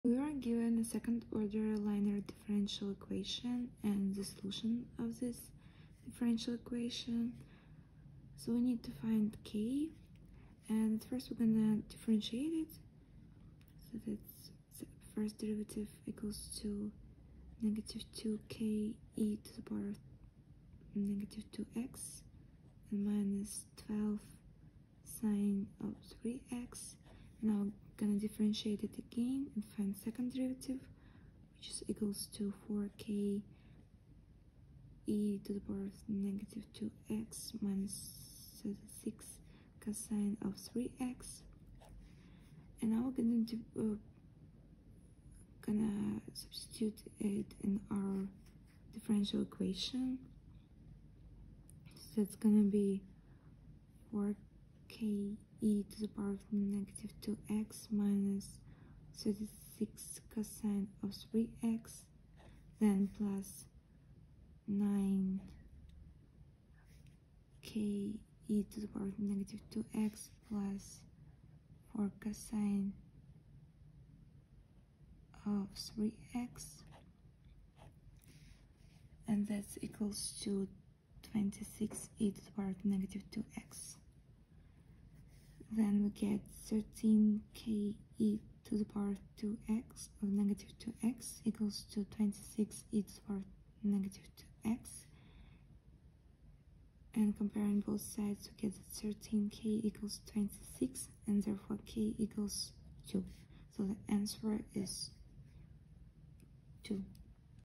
For what value of k, if any, will y=ke^{-2x}+4\cos(3x) be a solution to the ... | Plainmath
Solution to Differential equations question: For what value of k, if any, will y=ke^{-2x}+4\cos(3x) be a solution to the differential equation y" + 9y = 26e^{-2x}